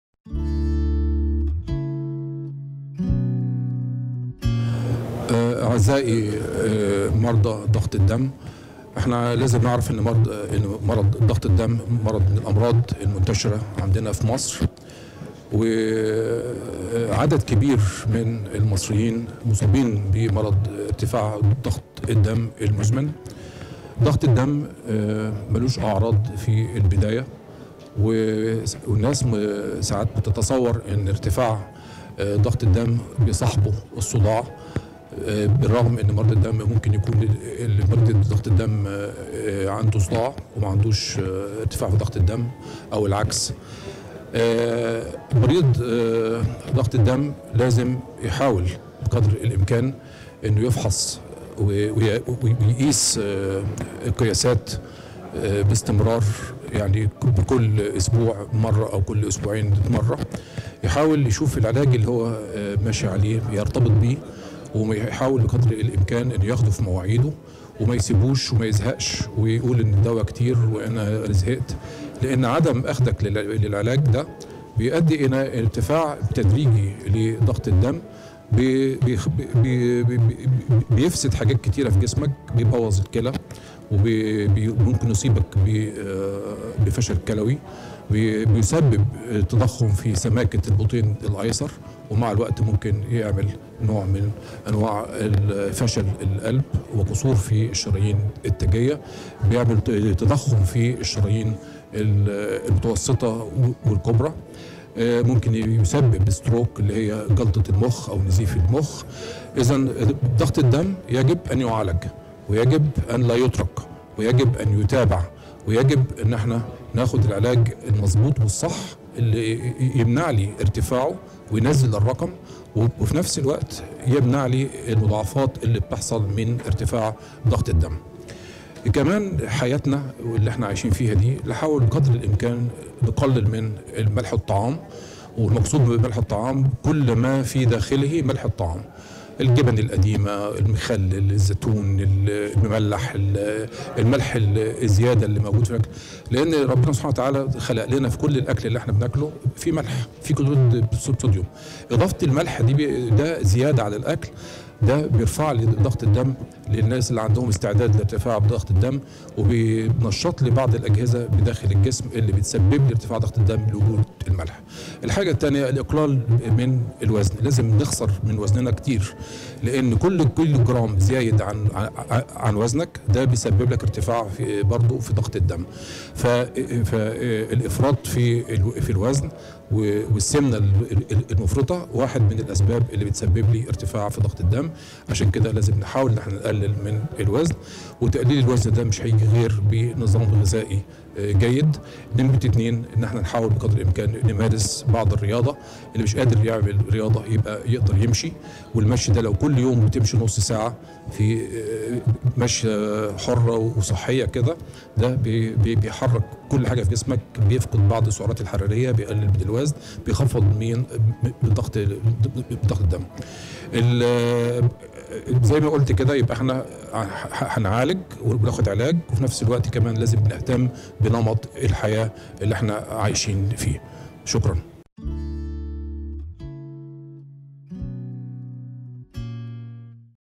كيفية التعامل مع إرتفاع ضغط الدم وخطورة عدم أخذ العلاج / دكتور عاطف البحرى
الدكتور عاطف البحرى
 أستشارى أمراض القلب بمستشفى بورسعيد العام
The Clinic